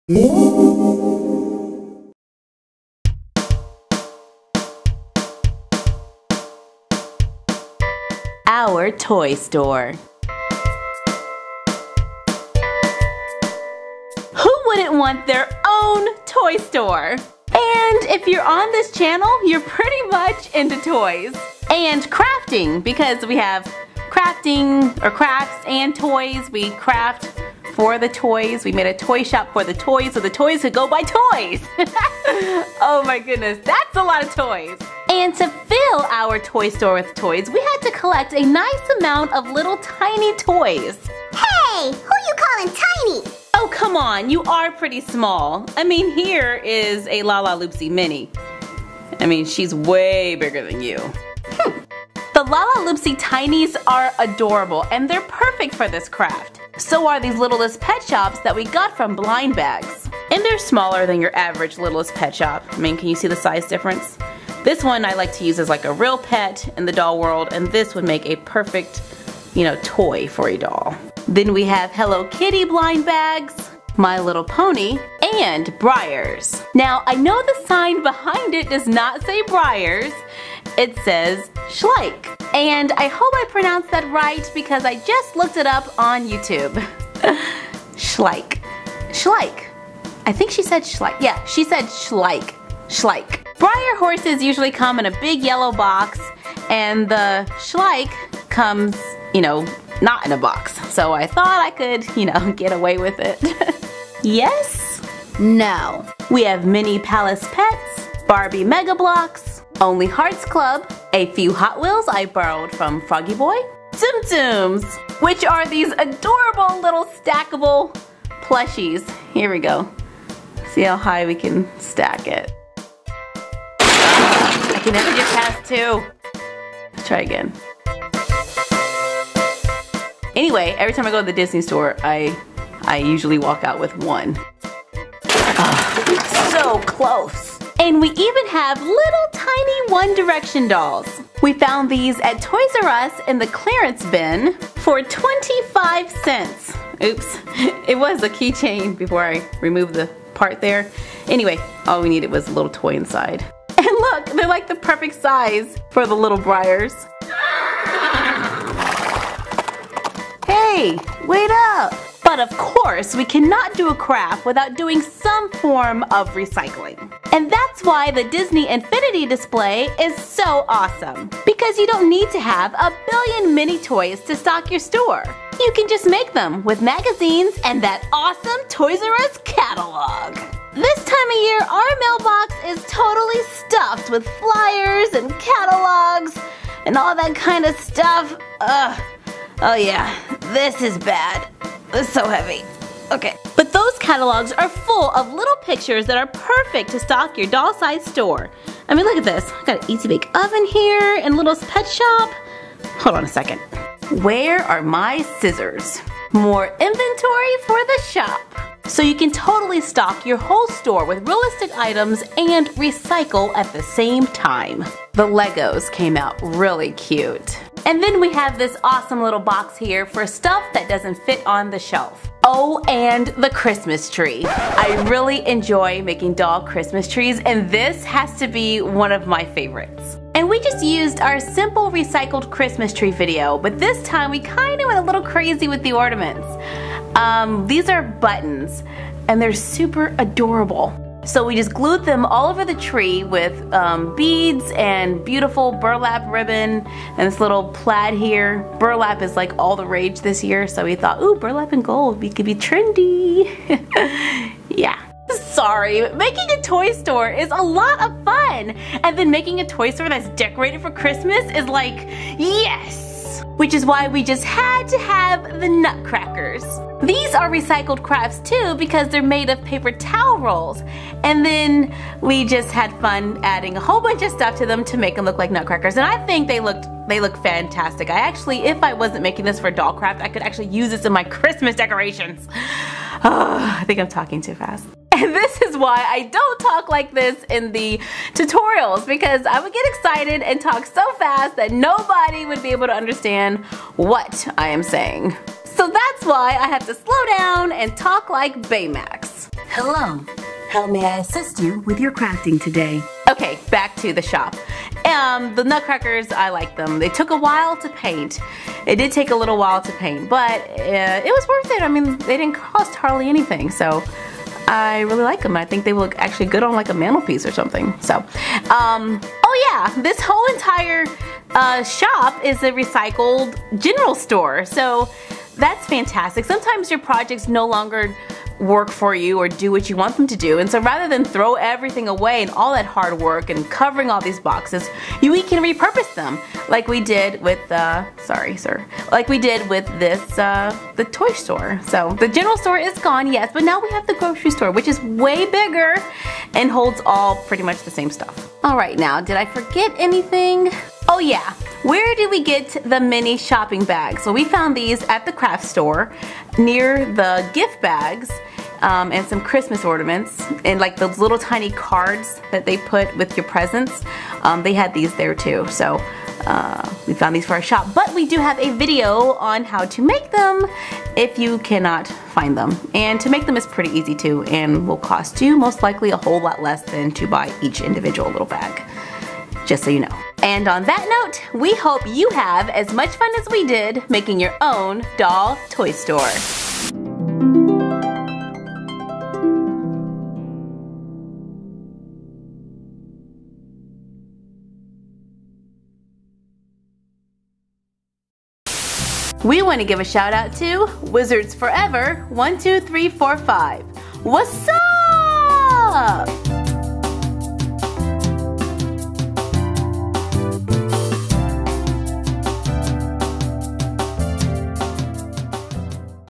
Fabulous Craft Review: Doll Toy Store
by request: Take a Fabulous behind the scenes look at our Fabsome Doll Toy Store!!